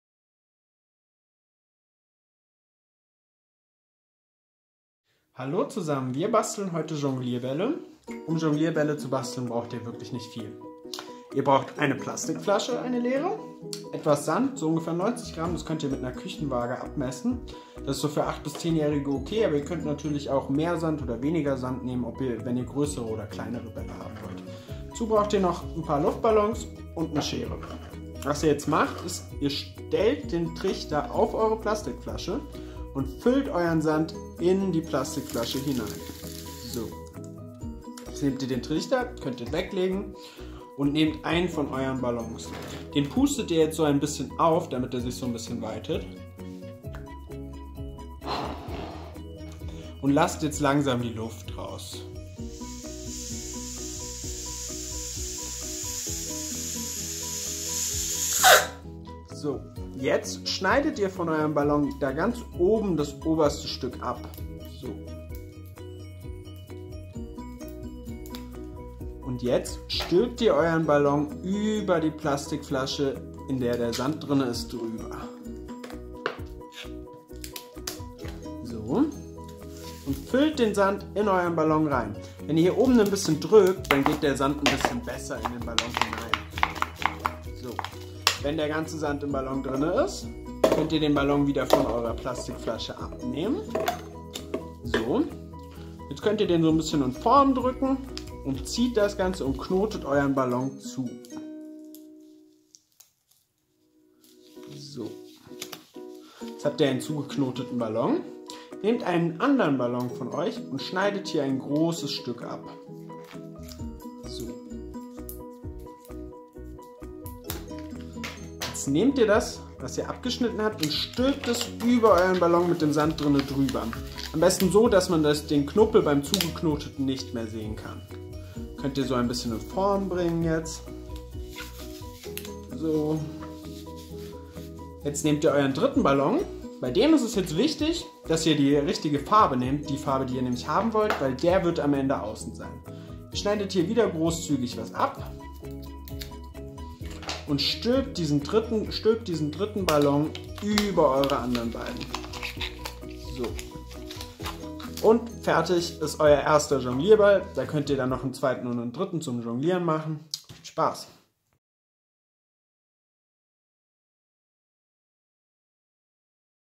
Jonglierbälle basteln
Aus Luftballons und Sand entstehen die Grundzutaten für großartige Jonglagen - eine weitere tolle Aktion aus dem Kinderclub